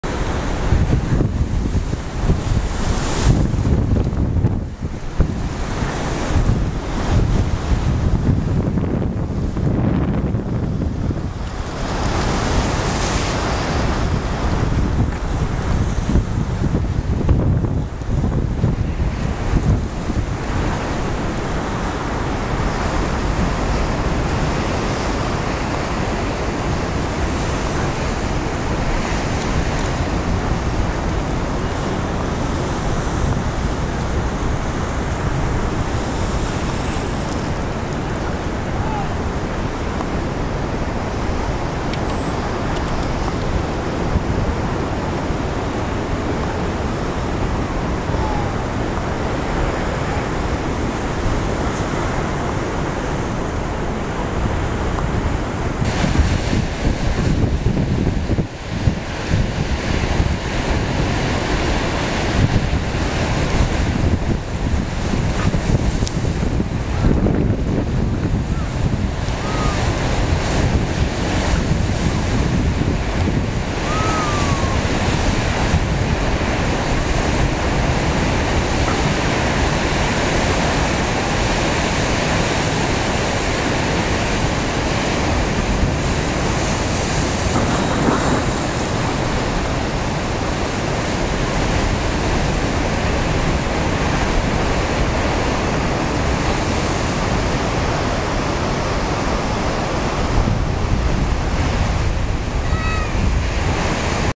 Unbelieveable Lake Waves (Milwaukee Bay) 10-31-14
With constant winds at 25+ mph on Lake Michigan, Milwaukee Bay was all churned up, with waves traveling in all directions. Although we were visitors to the area, based upon all the people taking pictures and videos the water conditions must have been very unusual (even for the locals)